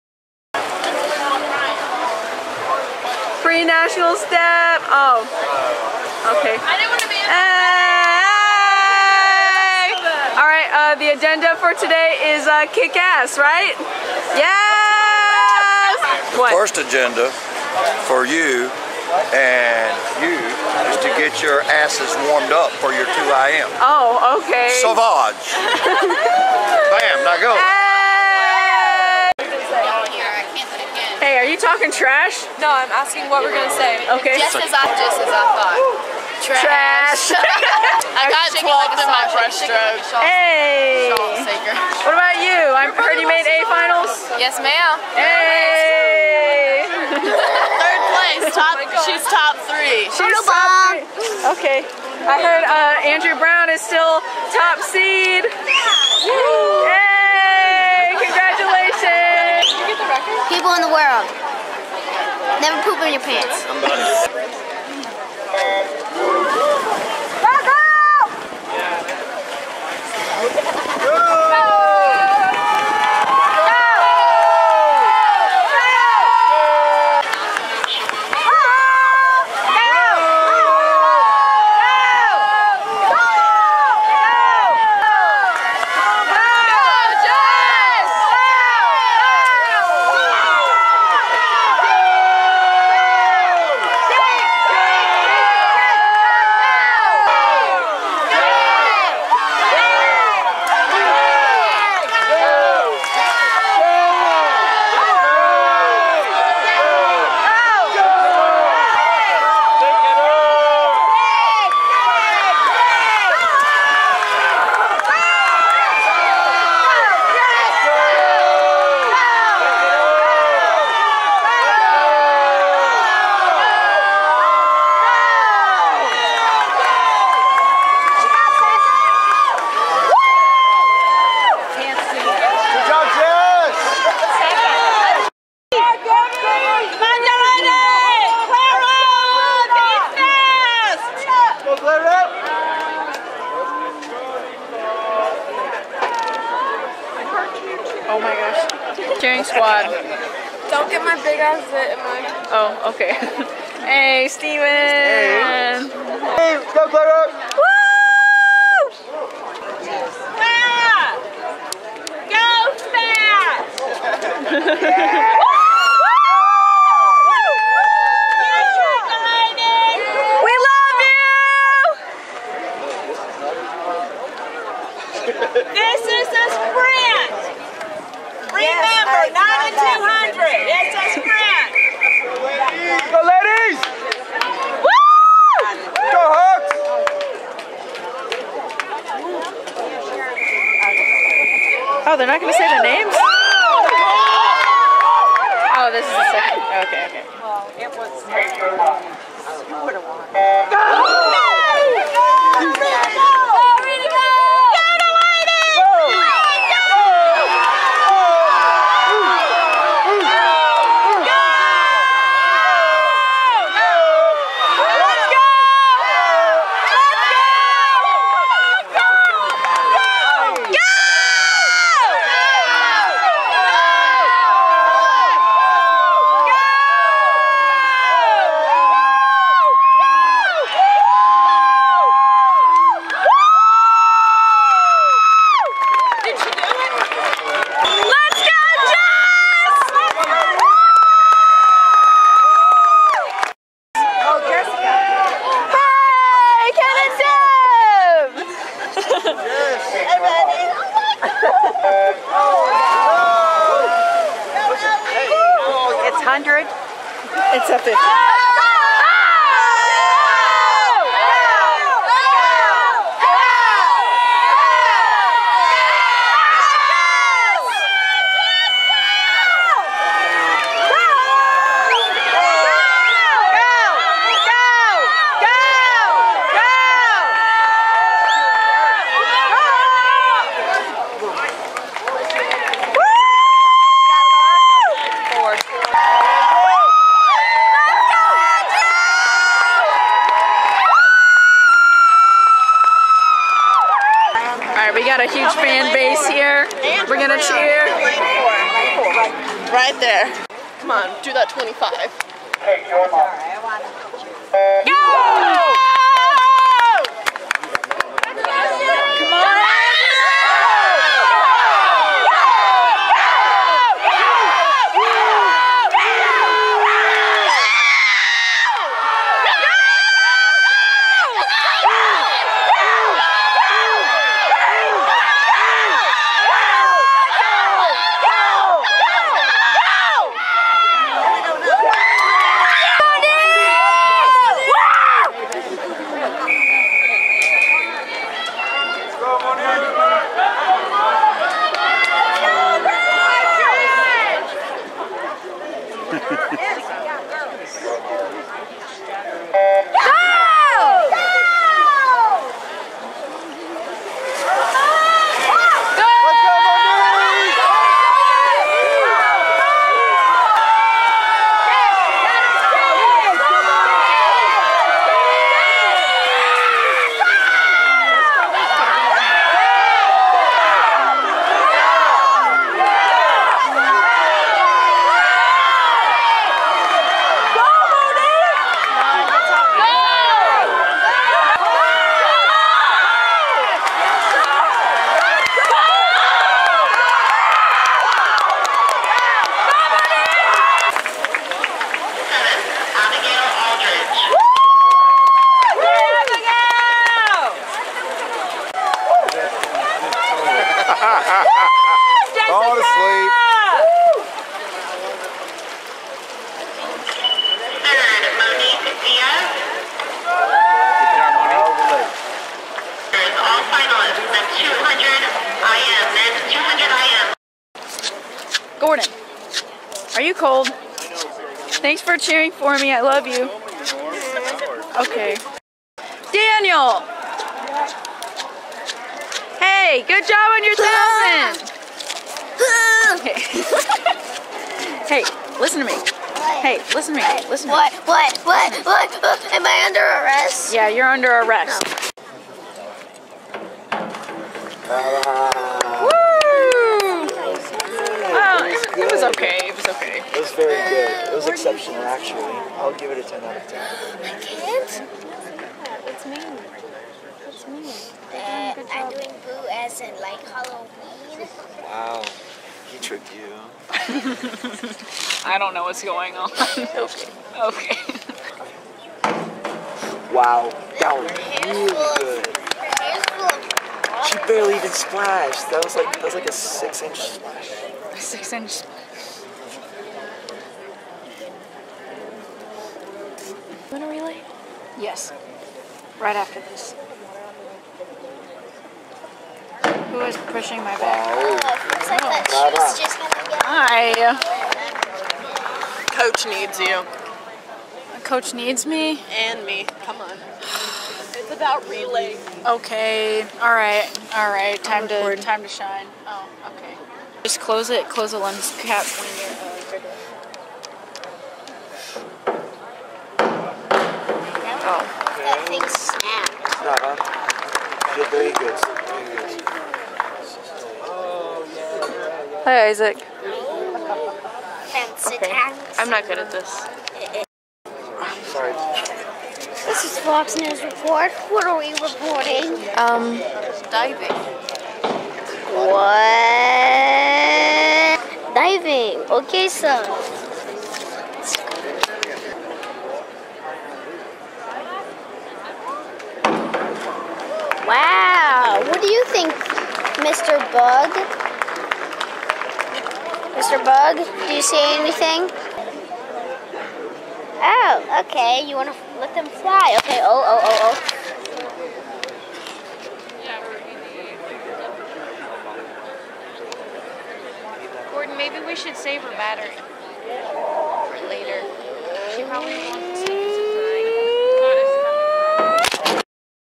Nationals Day 1
NJCAA National Swimming and Diving Championship, day 1. Swims include:
D. Smith, 50 BR
C. Simpsion, 50 BR
J. Graham, 50 BR
F. A. Brown, 50 BR
M. Henderson, 200 IM
A. Aldridge, 200 IM
J. Dunton, 200 IM.